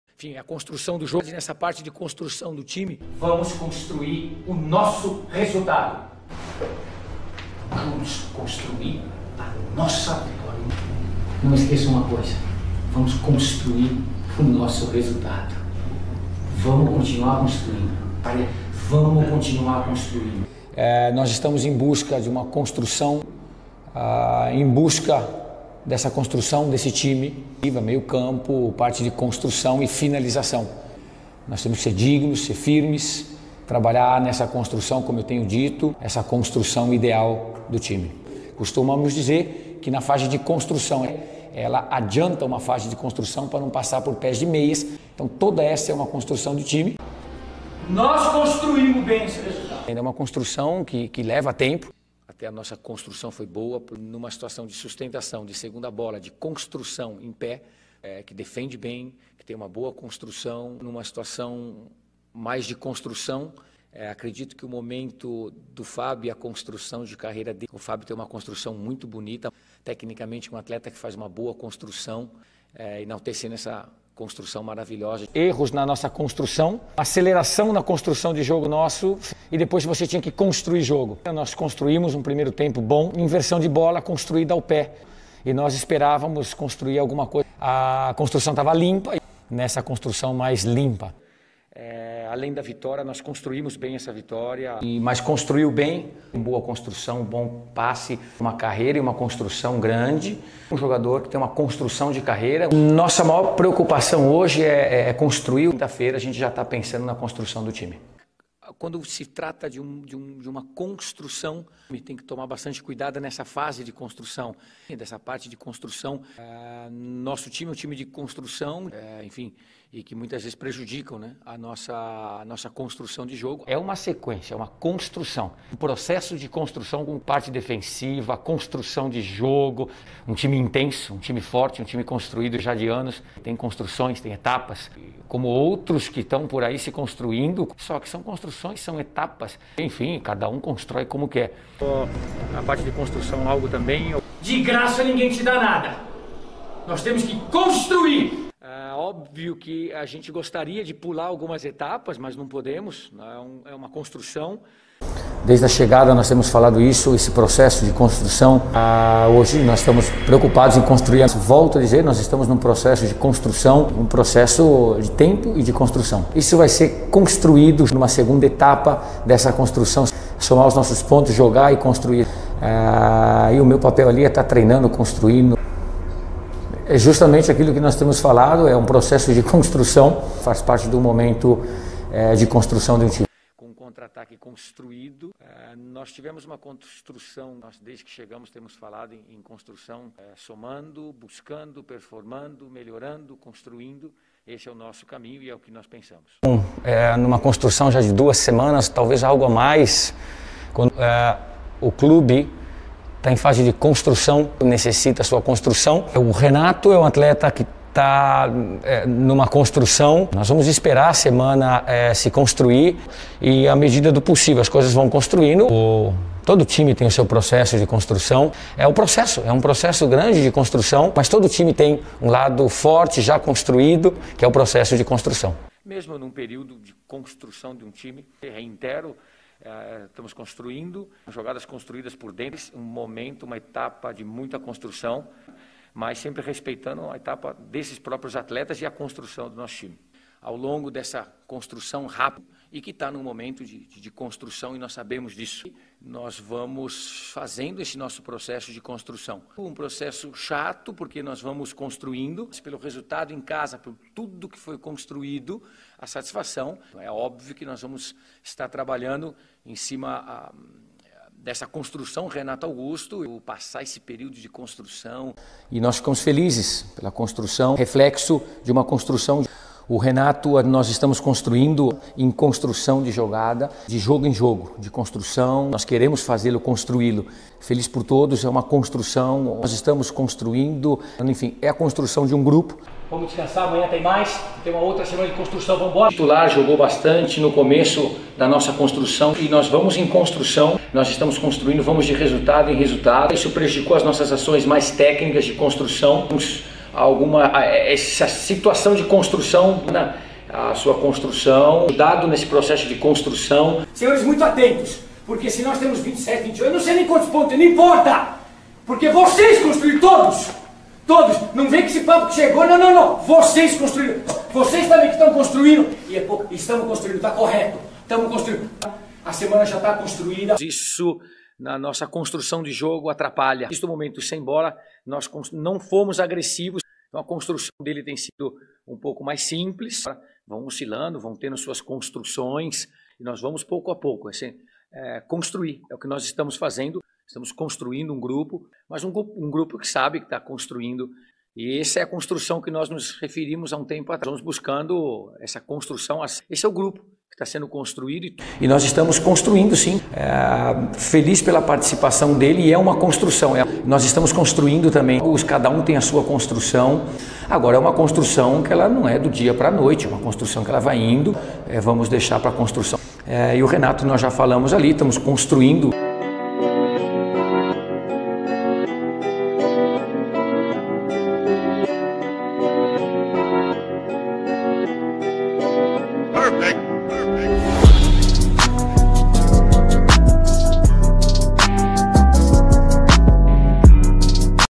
SYLVINHO, O CONSTRUTOR!!! - Todas as vezes que o Sylvinho falou a palavra "Construção" e derivados
O técnico Sylvinho do Corinthians fala muito o verbo "Construir" e derivados, e nesse vídeo você encontra um compilado com TODAS as vezes que ele falou isso em vídeo.

Fique com Deus S2